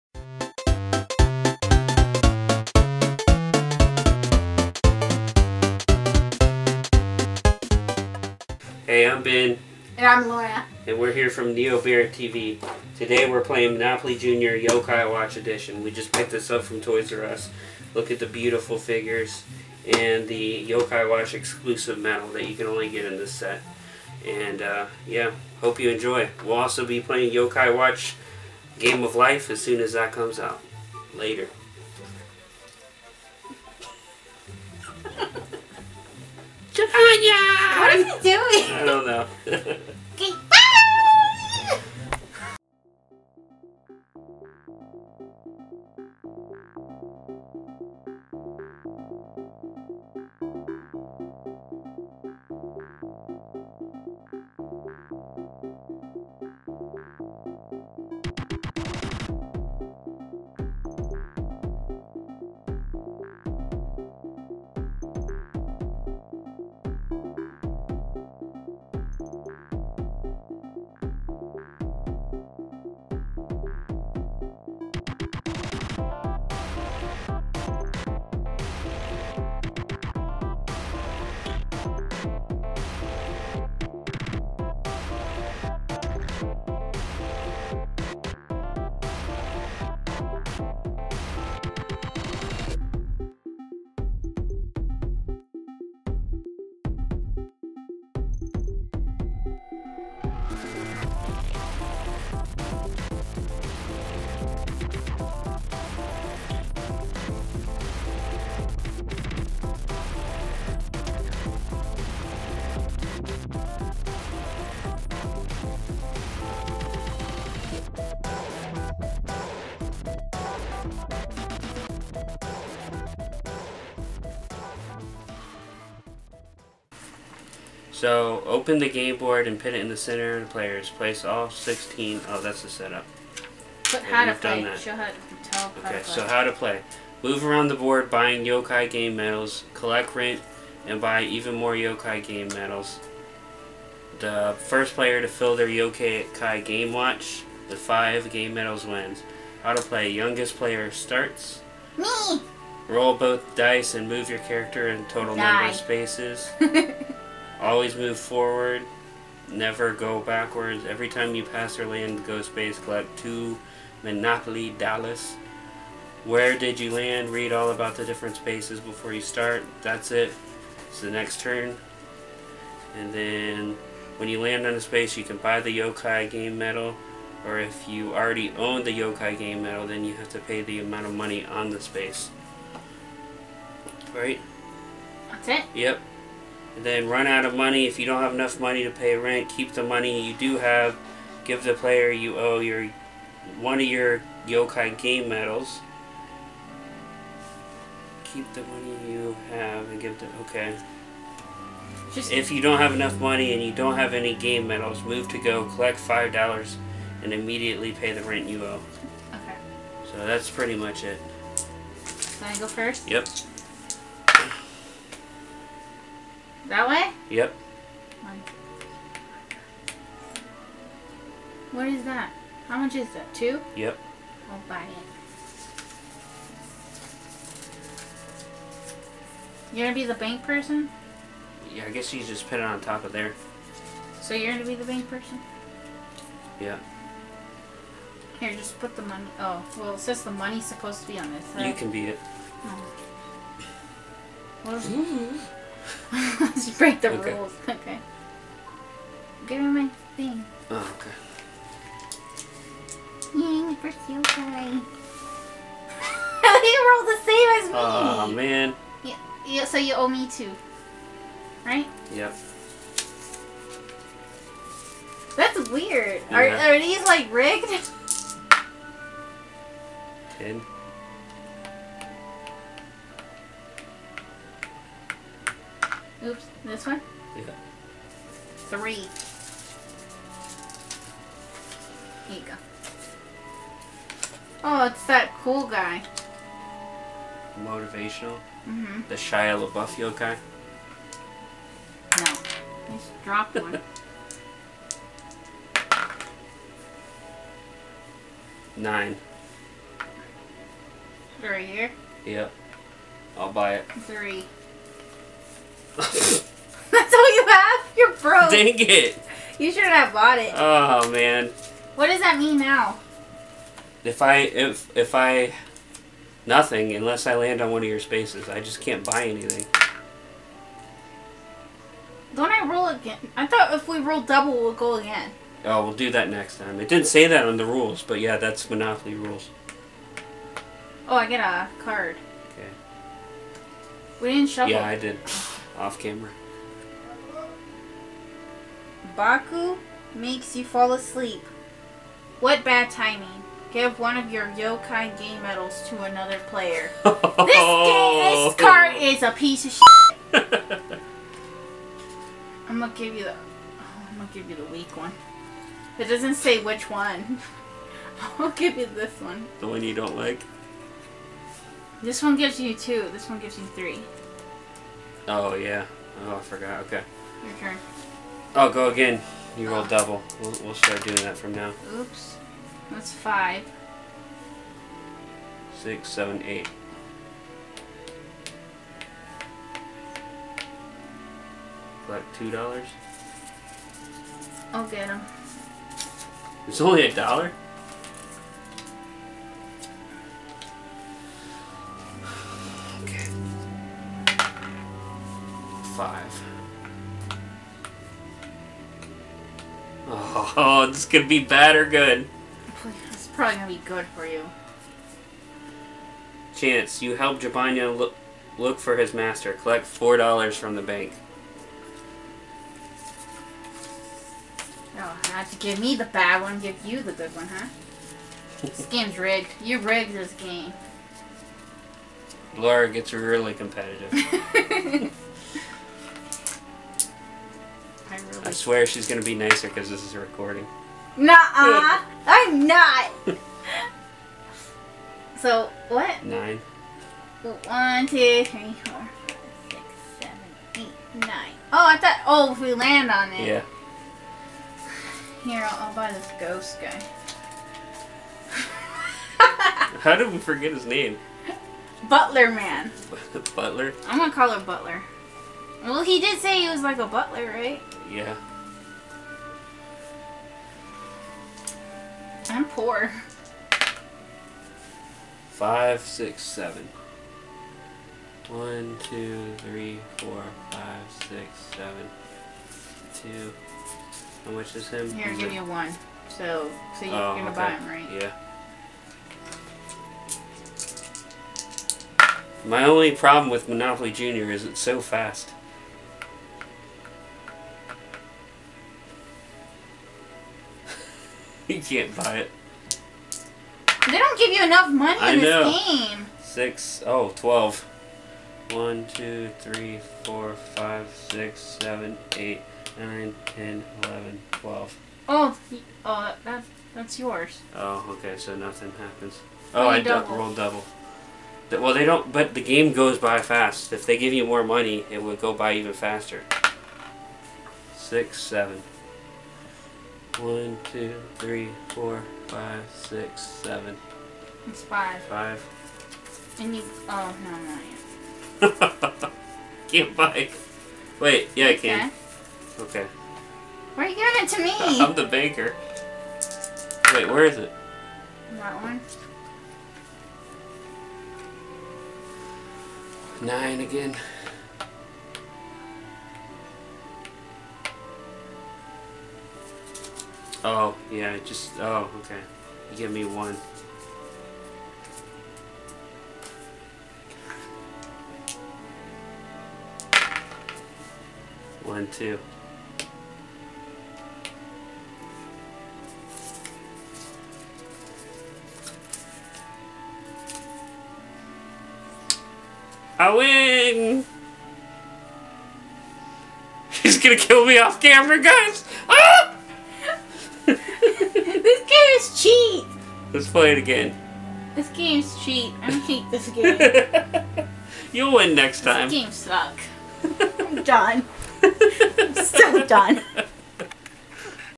Let's play Monopoly Junior: Yo-kai Watch Edition
Welcome to NeobearTV!

Ben and I have been going Yo-Kai CRAZY! Let's play some Monopoly Junior Yo-kai Watch Edition.